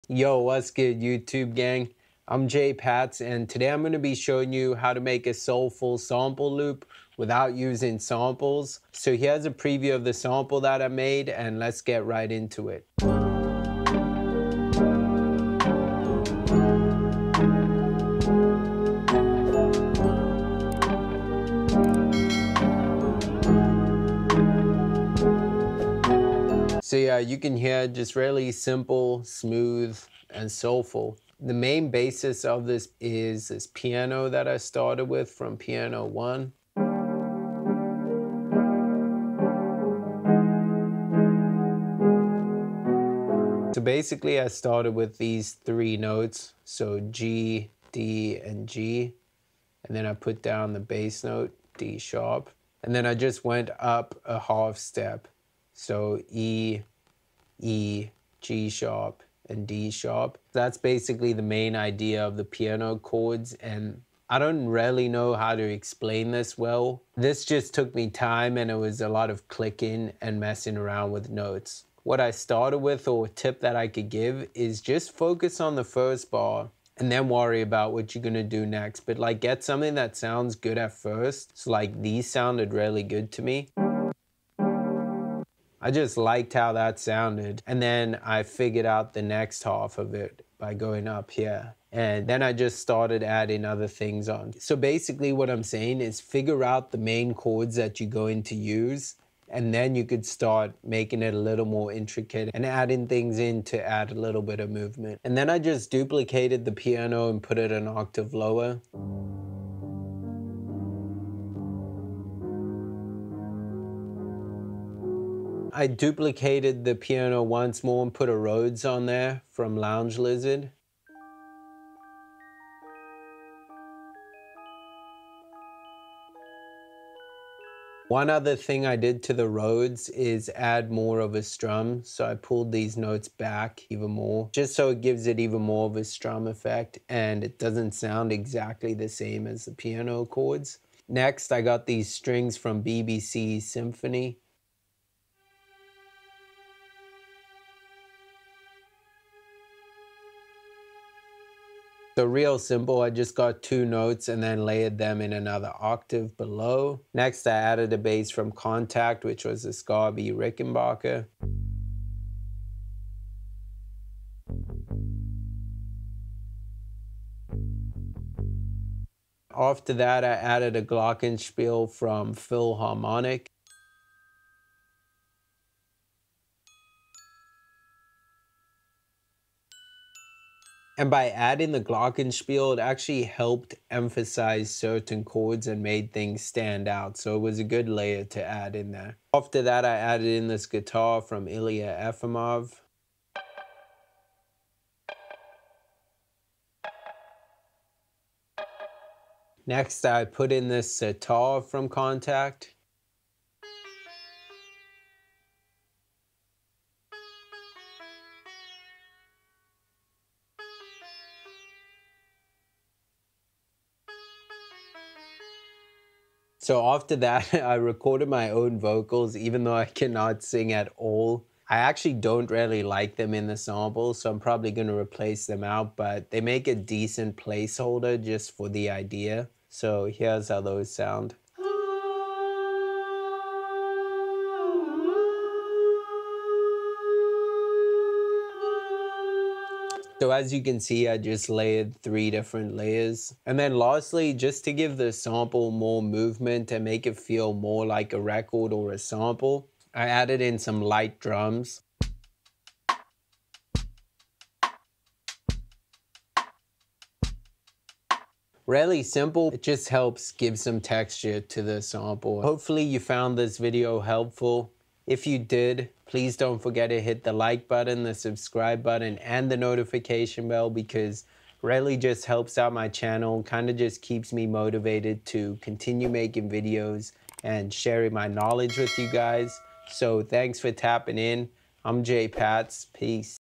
How to Make Soulful Samples From Scratch - Tutorial
In this video we will be diving into the world of vintage soul-inspired sample creation using VST's. In this tutorial, I will be using the powerful digital audio workstation, Ableton, alongside some of my favorite VST's including Piano One, BBC Symphony and Kontakt.

Through this video, you'll learn how to create stunning and soulful samples that can take your productions to the next level. We will cover sound selection and composition, with a focus on vintage-inspired sounds and techniques.

Whether you're a seasoned producer or just starting out, this tutorial will provide you with invaluable knowledge and skills that can be applied to your own productions. So, sit back, relax, and join me on this creative journey as we make some amazing vintage soul-inspired samples from scratch using VST's.

Connect with ya boi!
Time Stamps:
00:00 Intro
00:12 Sample preview
00:35 Piano
01:58 Rhodes
02:22 Strings
02:40 Bass
02:52 Glockenspiel
03:11 Guitar
03:22 Sitar
03:38 Vocals
04:11 Drums
04:22 Outro